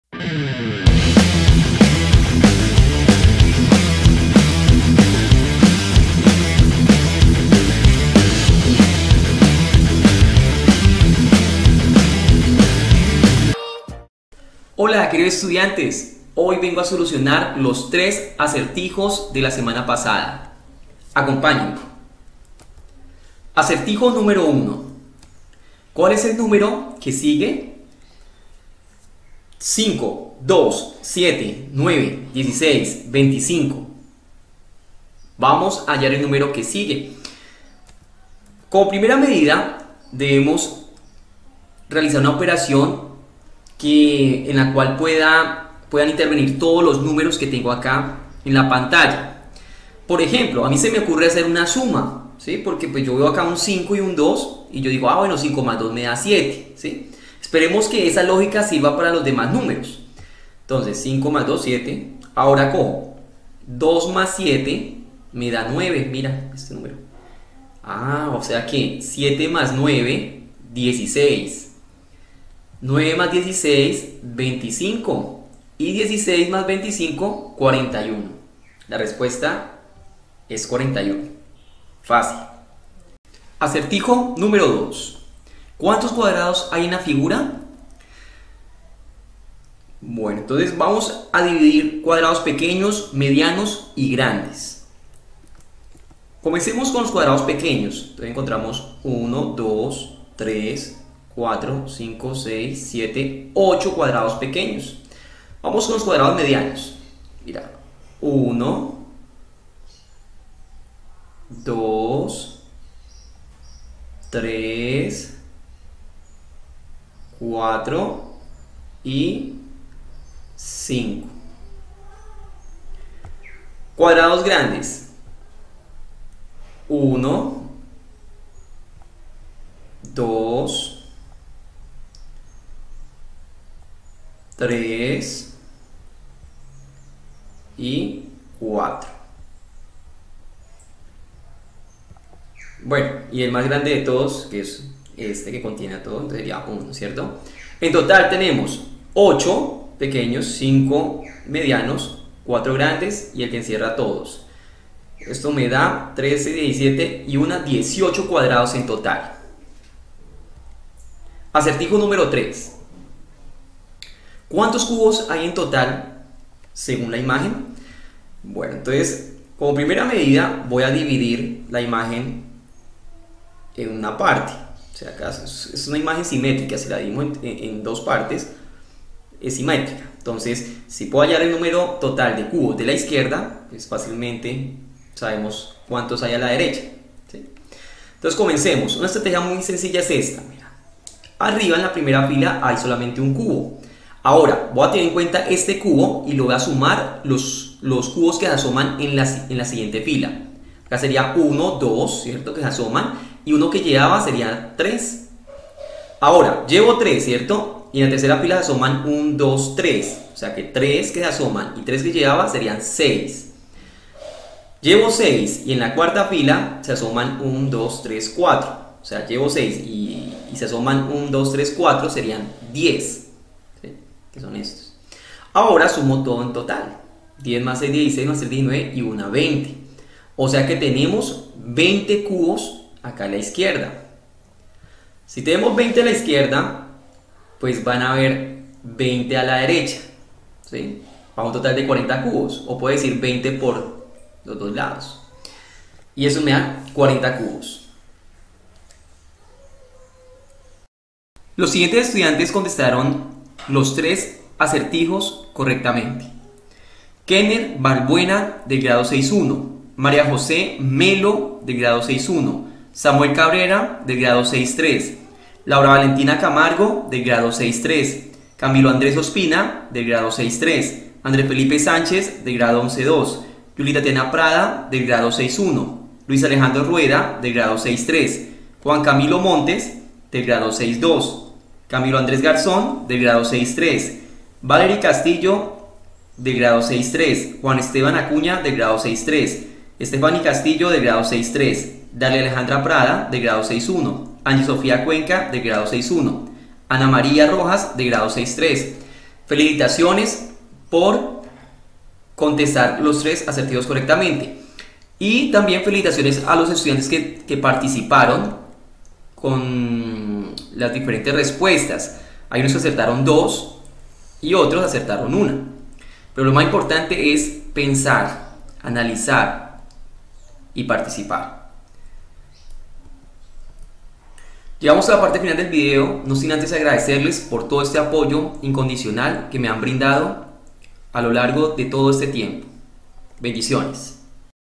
Solución de los 3 acertijos
Acertijo uno
¿Qué número falta?
5,2,7,9,16,25, ?
Acertijo dos
¿Cuántos cuadrados hay en la imagen?
¿Cuántos cubos hay en la imagen?